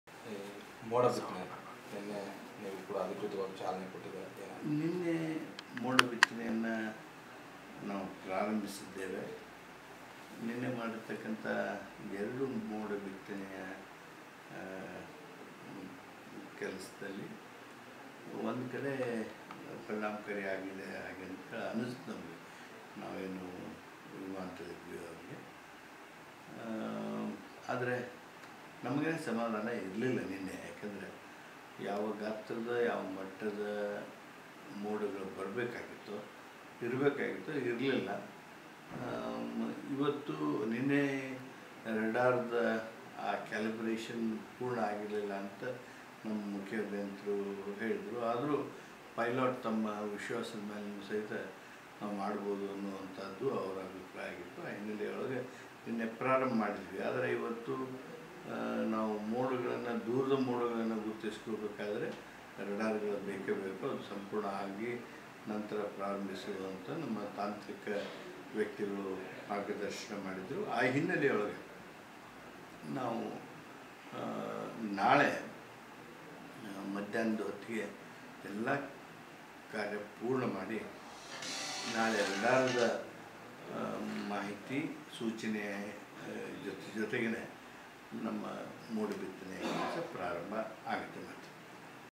H K PATIL Cloud Seeding Speech
ಮೋಡ ಭಿತ್ತನೆ ಕಾರ್ಯಕ್ರಮ

ನಿನ್ನೆ ಮೋಡ ಭಿತ್ತನೆಯನ್ನ ನಾವು ಪ್ರಾರಂಭಿಸಿದ್ದೇವೆ ನಿನ್ನೆ ಮಾಡಿರುವ  ಎರಡು ಮೋಡ ಭಿತ್ತನೆಯ  ಕೆಲಸದಲ್ಲಿ ಒಂದು ಕಡೆ ಪರಿಣಾಮಕಾರಿಯಾಗಿದೆ ಎಂದು ನಮಗೆ ಅನ್ನಿಸಿದೆ.  ಆದರೆ ನಮಗೆ ಅಷ್ಟು ಸಮಾಧಾನ ಸಿಕ್ಕಿಲ್ಲ ನಿನ್ನೆ  ಏಕೆಂದರೆ ಯಾವ ಗಾತ್ರದ ಯಾವ ಮಟ್ಟದ  ಮೋಡಗಳು ಬರಬೇಕಿತ್ತು, ಇರಬೇಕಿತ್ತು ಆ ಮೋಡಗಳು ಇರಲಿಲ್ಲ ನಿನ್ನೆ ರಾಡಾರದ  ಕ್ಯಾಲಿಬರೇಷನ್  ಪೂರ್ಣವಾಗಿರಲಿಲ್ಲ ಎಂದು ಮುಖ್ಯ ಅಭ್ಯಂತರರು ಹೇಳಿದರು. ಆದರೂ ಕೂಡ  ಪೈಲೇಟ್  ವಿಶ್ವಾಸ ಸಹಿತ ನಾವು ಮಾಡಬಹುದು ಎಂಬುದು ಅವರ ಅಭಿಪ್ರಾಯವಾಗಿತ್ತು ಆದರೆ ನಿನ್ನೆ ಪ್ರಾರಂಭ ಮಾಡಿದ್ದೇವೆ ಆದರೆ ಇವತ್ತು ನಾವು ಮೋಡಗಳನ್ನ ದೂರದ ಮೋಡಗಳನ್ನ ಗುರಿತಿಸಿಕೊಳ್ಳಬೇಕಾದರೆ ರಾಡಾರಗಳು ಬೇಕೆ ಬೇಕು ಅದು ಸಂಪೂರ್ಣ  ಸರಿಯಾಗಿ ನಂತರ ಪ್ರಾರಂಭಿಸಬೇಕು ಎಂದು ನಮ್ಮ ತಾಂತ್ರೀಕ ವ್ಯಕ್ತಿಗಳು ಮಾರ್ಗದರ್ಶನ ನೀಡಿದ್ದಾರೆ.
 ಆ ಹಿನ್ನೆಲೆಯಿಂದ ನಾವು ನಾಳೆ ಮಧ್ಯಾಹ್ನದ ವೇಳೆಗೆ ರಾಡಾರದ ಕಾರ್ಯ ಪೂರ್ಣ  ಮಾಡಿ ಮಾಹಿತಿ ಸೂಚನೆ ಜೊತೆ ಜೊತೆಗೆ  ಯೋಜನೆ ಮತ್ತೆ ಪ್ರಾರಂಭವಾಗುತ್ತದೆ ಎಂದು ಗ್ರಾಮೀಣಾಭಿವೃದ್ಧಿ ಮತ್ತು ಪಂಚಾಯತ್ ರಾಜ್ ಸಚಿವರಾದ ಮಾನ್ಯ ಎಚ್.ಕೆ ಪಾಟೀಲರು ಹೇಳಿದರು.